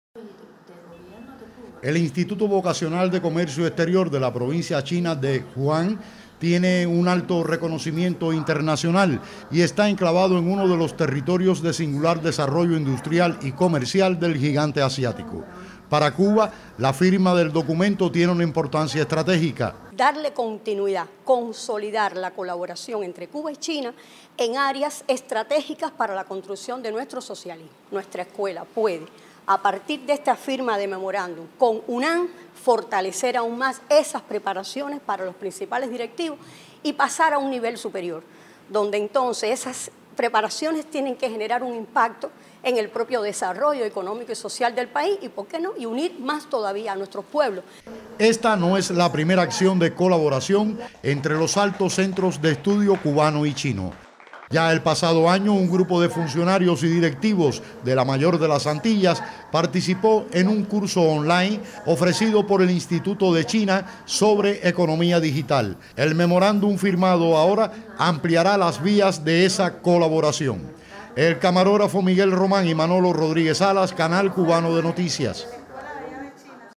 Cuba y China colaboración en la formación de cuadros
CUBA Y CHINA COOPERACION EN FORMACION DE CUADROS

CUBA Y CHINA DIERON ESTE LUNES UN NUEVO PASO DE ACERCAMIENTOEN LA PREPARACIÓN DE LOS CUADROS DE LA ADMINISTRACIÓN PÚBLICA DEL SECTOR EMPRESARTIAL, CON LA FIRMADEUN MEMORANDUM DE COLABORACIÓN ENTRE EL INSTITUTO VOCACIONAL DE COMERCIO EXTERIOR DE LAPROVINCIA  DE HUAN Y LA ESCUELA SUPERIOR DE CUADROS DEL ESTADO Y EL GOBIERNO CON SEDE EN LA HABANA...

RUBRICARON EL DOCUMENTO EL SEÑOR  TANG JIU, PRESIDENTE DEL INSTITUTO VOCACIONAL DE COMERCIO EXTERIOR DE LA PROVINCIA HUAN, Y LA DOCTORA MERCEDES DELGADO FERNÁNDEZ, RECTORA DE LA ESCUELA SUPERIOR DE CUADROS DEL ESTADO Y EL GOBIERNO DE CUBA...

GENERADOR

1- DRA. MERCEDES DELGADO FERNÁNDEZ, RECTORA DE LA ESCUELA SUPERIOR DE CUADROS DEL ESTADO Y DEL GOBIERNO